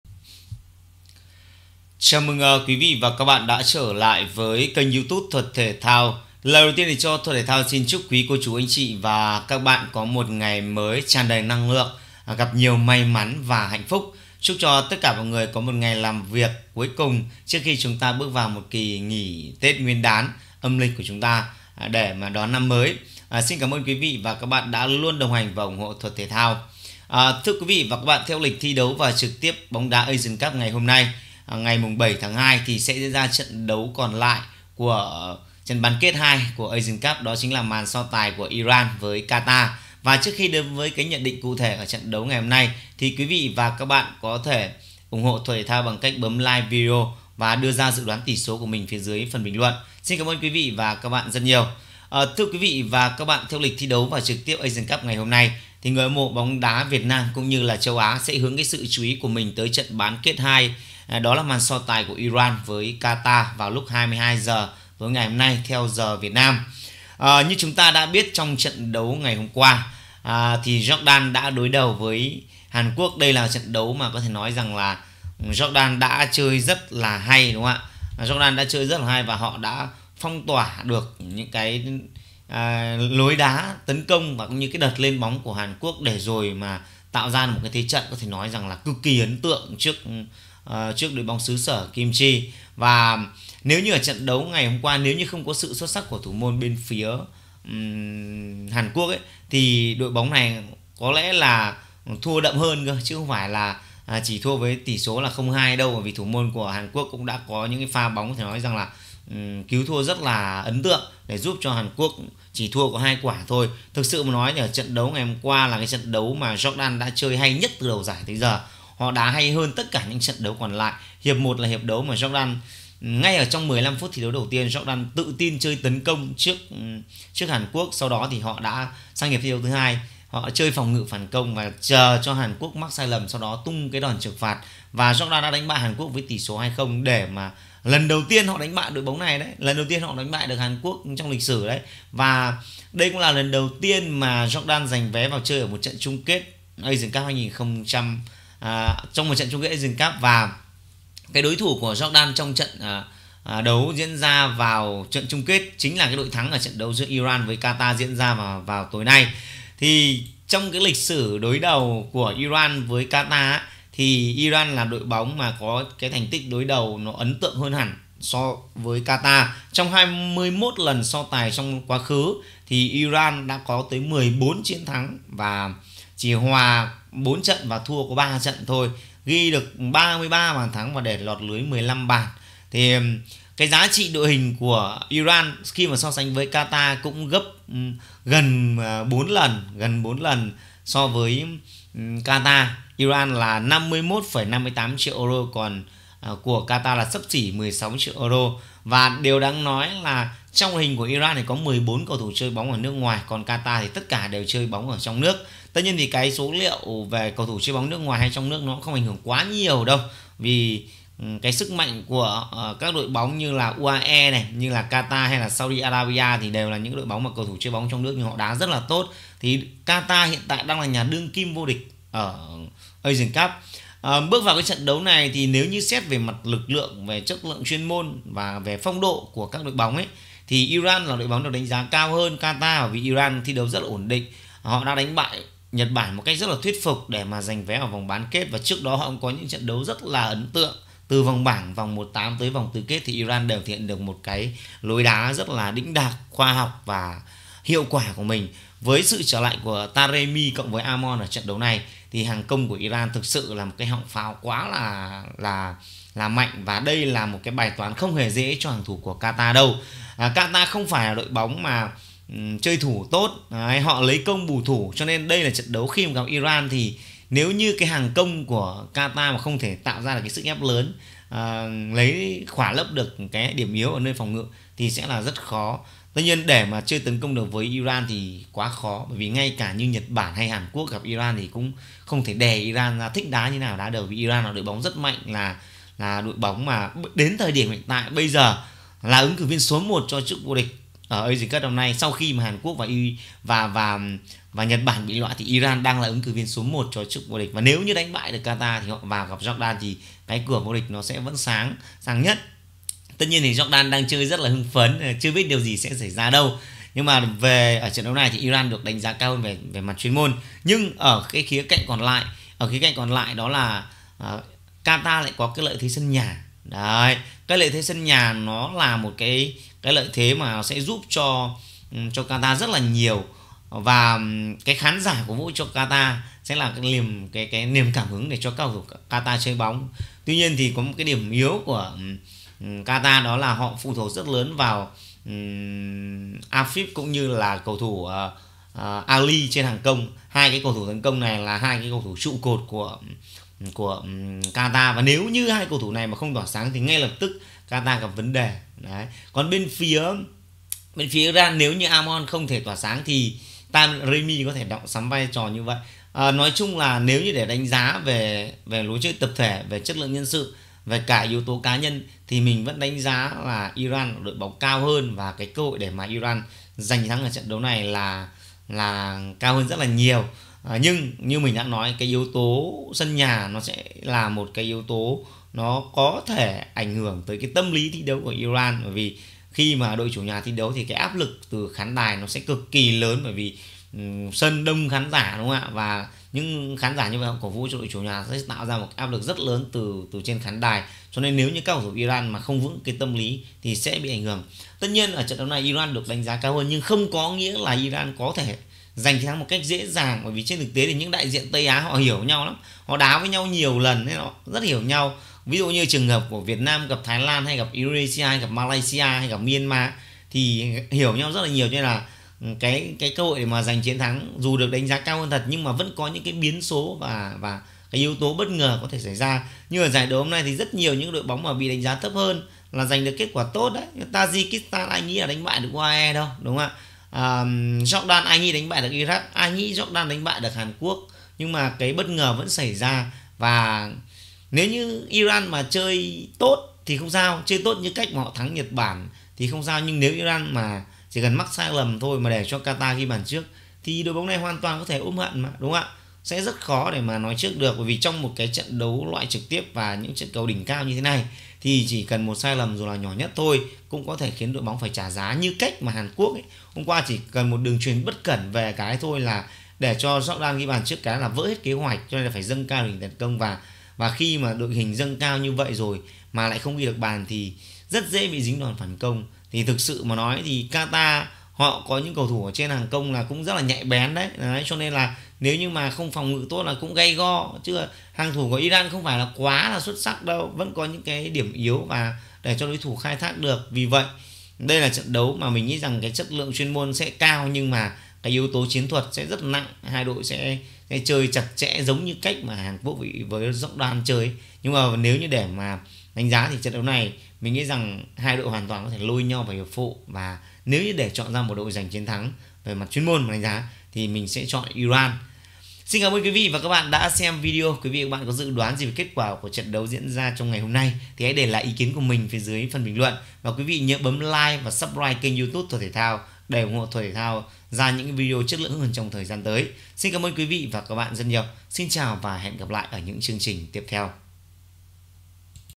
Lịch trực tiếp bóng đá Asian Cup 2023 hôm nay 7-2 | Iran vs Qatar bán kết trên VTV5 và FPT Play
Lịch thi đấu trực tiếp bóng đá Asian Cup 2023 hôm nay 7-2 | Iran vs Qatar bán kết trên VTV5 và FPT Play diễn ra vào lúc 22h tối nay để tranh vé vào chung kết.
✔ Trang youtube chính thức của nhà báo Dương Thuật